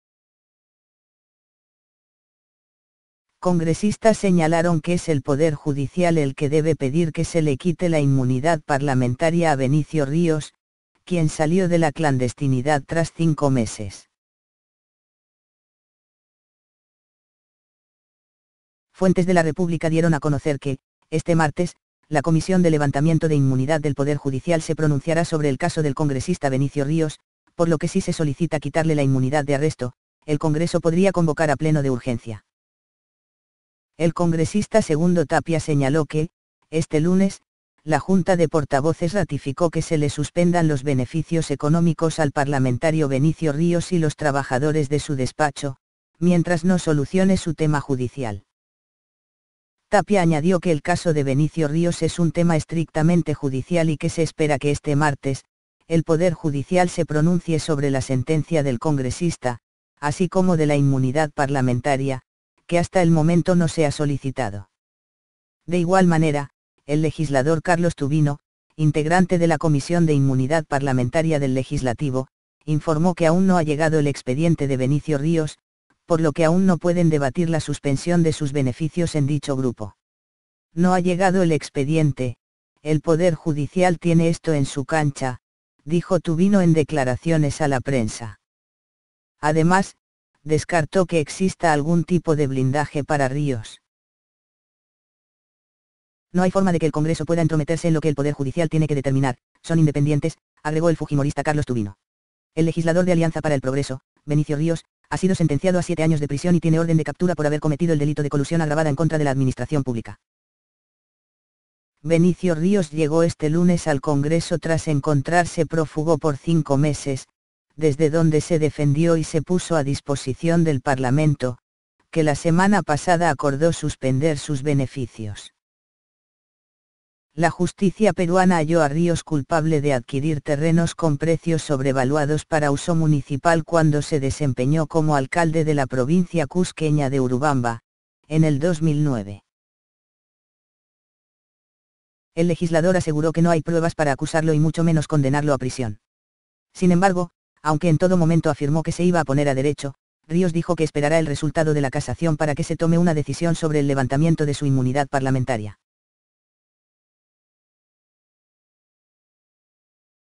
Poder Judicial sigue sin pedir levantamiento de inmunidad parlamentaria de Benicio Ríos
"Congresistas señalaron que es el Poder Judicial el que debe pedir que se le quite la inmunidad parlamentaria a Benicio Ríos, quien salió de la clandestinidad tras 5 meses.
Fuentes de La República dieron a conocer que, este martes, la comisión de levantamiento de inmunidad del Poder Judicial se pronunciará sobre el caso del congresista Benicio Ríos, por lo que si se solicita quitarle la inmunidad de arresto, el Congreso podría convocar a Pleno de urgencia.
El congresista Segundo Tapia señaló que, este lunes, la Junta de Portavoces ratificó que se le suspendan los beneficios económicos al parlamentario Benicio Ríos y los trabajadores de su despacho, mientras no solucione su tema judicial.
Tapia añadió que el caso de Benicio Ríos “es un tema estrictamente judicial” y que se espera que este martes, el Poder Judicial se pronuncie sobre la sentencia del congresista, así como de la inmunidad parlamentaria, que hasta el momento no se ha solicitado.
De igual manera, el legislador Carlos Tubino, integrante de la Comisión de Inmunidad Parlamentaria del Legislativo, informó que aún no ha llegado el expediente de Benicio Ríos, por lo que aún no pueden debatir la suspensión de sus beneficios en dicho grupo.
""No ha llegado el expediente, el Poder Judicial tiene esto (el caso) en su cancha"", dijo Tubino en declaraciones a la prensa. Además, descartó que exista algún tipo de blindaje para Ríos.
""No hay forma de que el Congreso pueda entrometerse en lo que el Poder Judicial tiene que determinar, son independientes"", agregó el fujimorista Carlos Tubino.
El legislador de Alianza para el Progreso, Benicio Ríos, ha sido sentenciado a siete años de prisión y tiene orden de captura por haber cometido el delito de colusión agravada en contra de la administración pública.
Benicio Ríos llegó este lunes al Congreso tras encontrarse prófugo por cinco meses, desde dónde se defendió y se puso a disposición del Parlamento, que la semana pasada acordó suspender sus beneficios.
La justicia peruana halló a Ríos culpable de adquirir terrenos con precios sobrevaluados para uso municipal cuando se desempeñó como alcalde de la provincia cusqueña de Urubamba, en el 2009.
el legislador aseguró que no hay pruebas para acusarlo y mucho menos condenarlo a prisión. Sin embargo, aunque en todo momento afirmó que se iba a poner a derecho, Ríos dijo que esperará el resultado de la casación para que se tome una decisión sobre el levantamiento de su inmunidad parlamentaria.